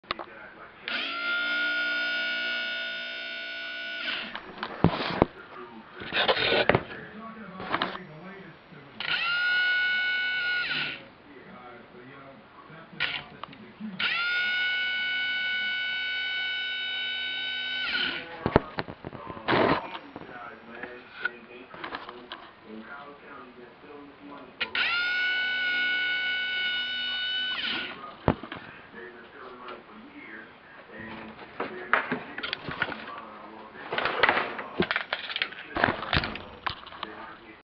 12 volt boat bilge pump
testing a 12 boat bilge pump.